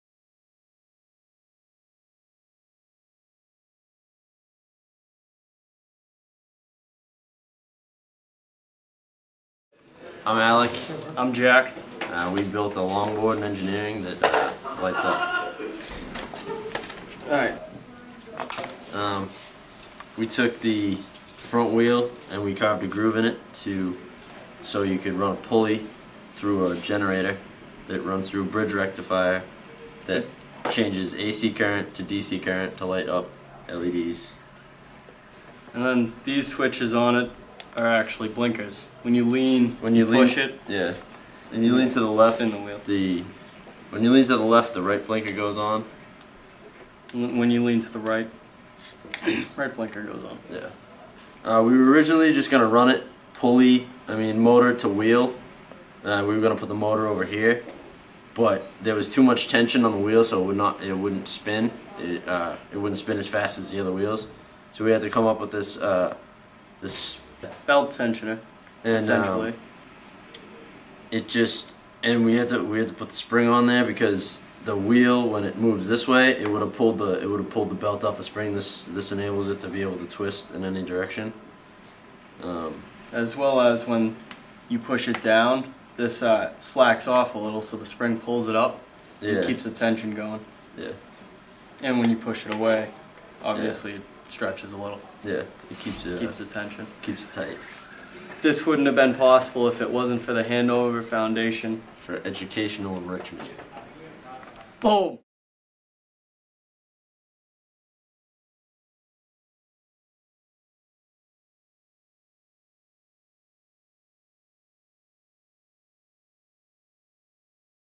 Alternative Energy with a Longboard
This project was done in Engineering 3 class by Jack Mahoney & Alec LaCroix as an Independent Study in Alternative Energy.
Produced by: Madison Plummer